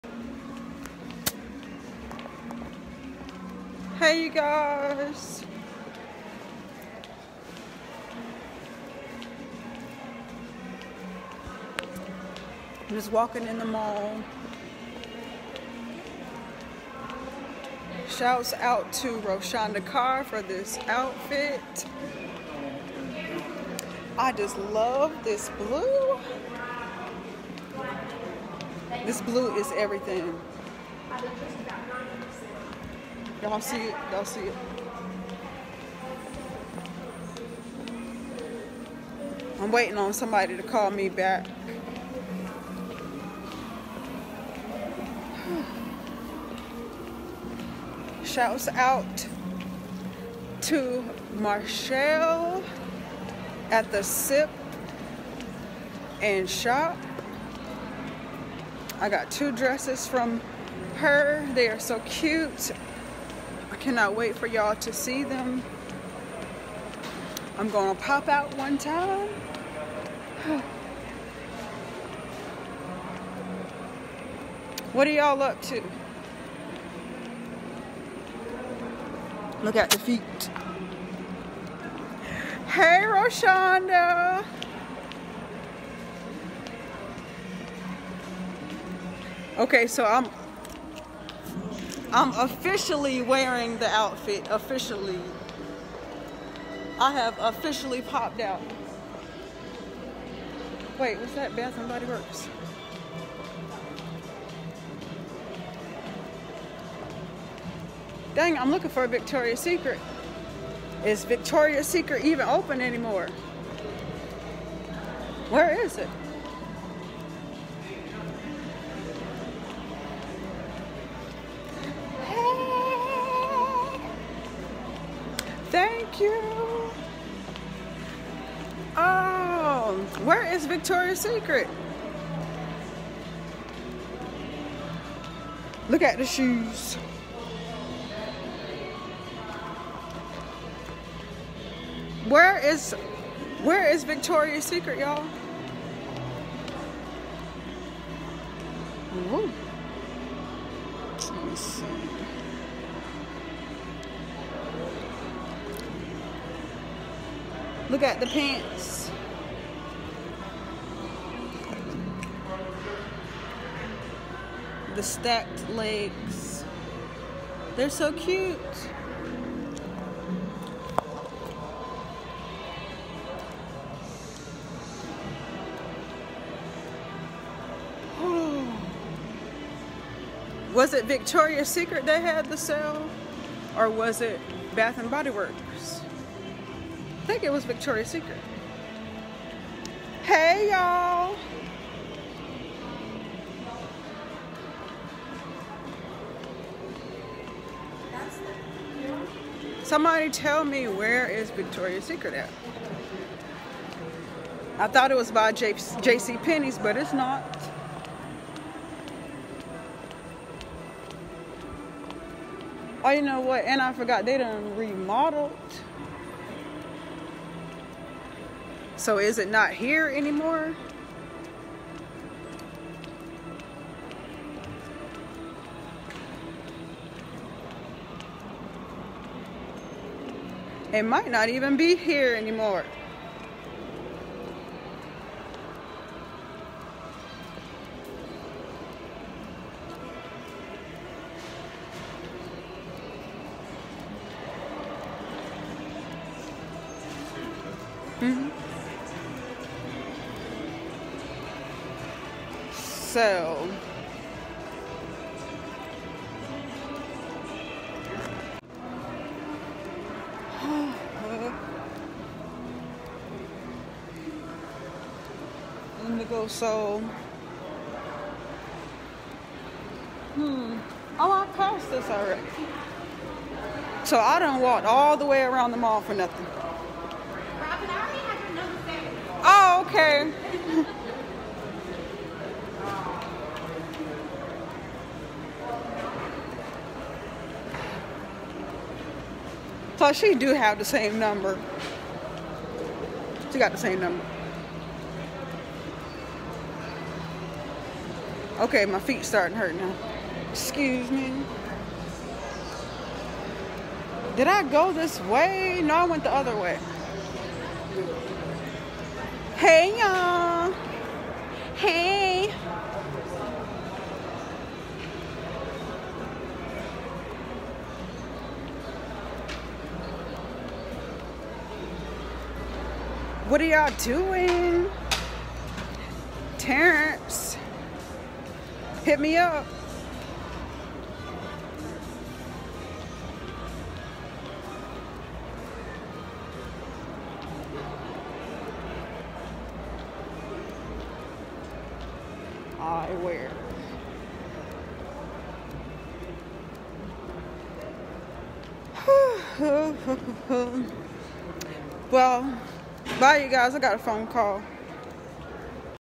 WALKING AROUND IN THE MALL
The mall has been remodeled and some stores did not return🥲